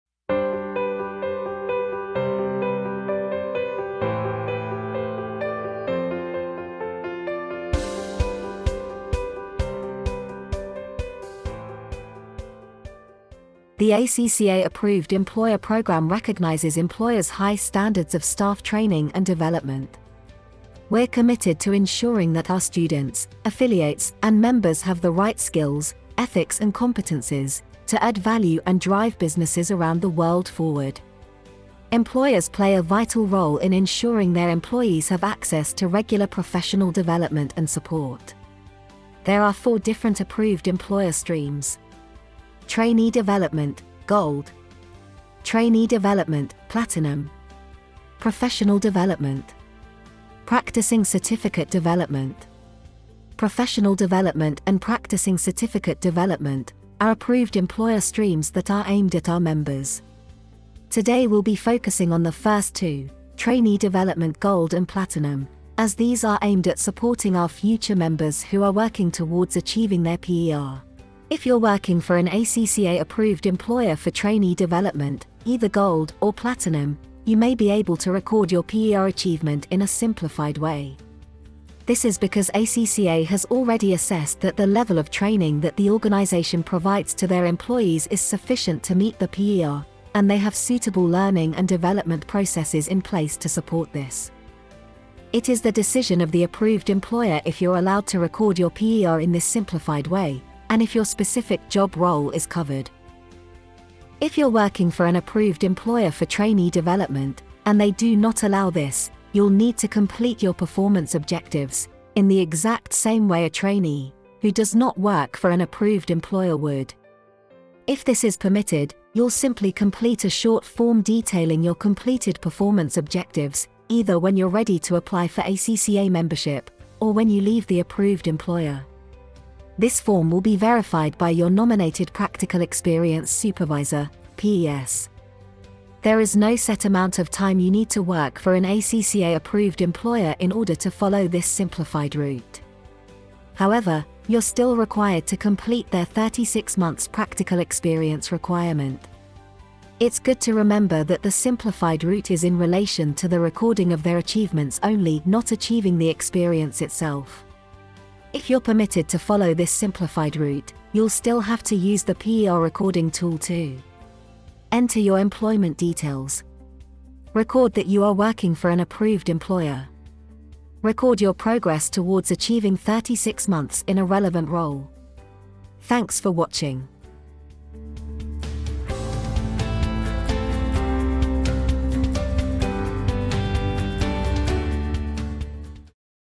Working for an Approved Employer
If you are working for an Approved Employer for Trainee Development, you may record your practical experience in a simplified way. This video highlights these differences and how you would complete some, or all, of your PER with this employer.